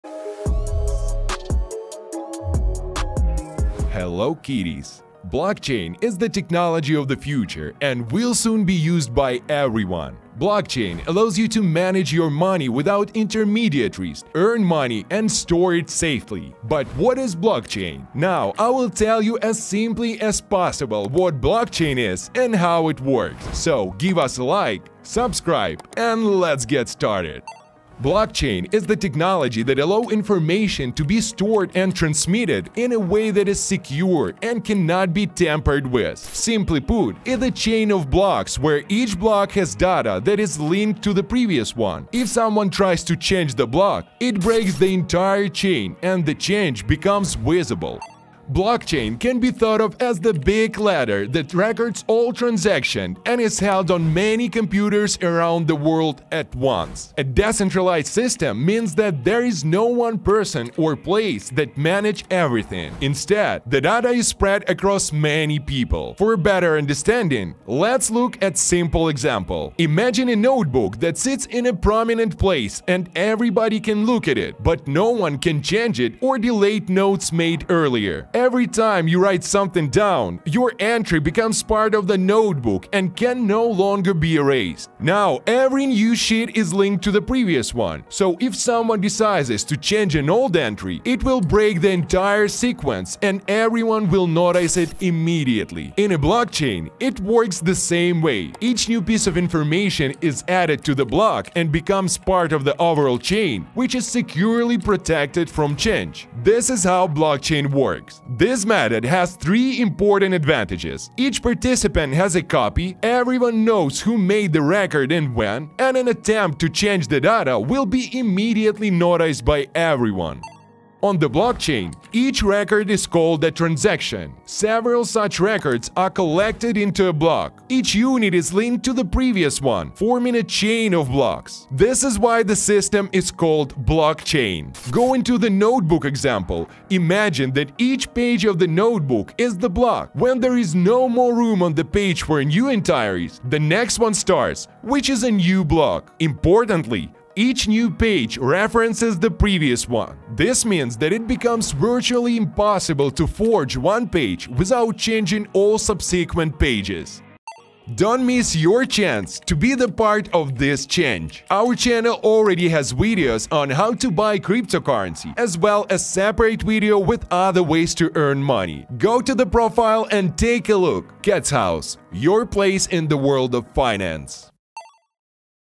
How Blockchain ACTUALLY Work | A Simple Explanation For Beginners | PART 1
- Play in CATS 🎁 There's a first-time gift!

In this video we talk about How Blockchain ACTUALLY Work | A Simple Explanation For Beginners | PART 1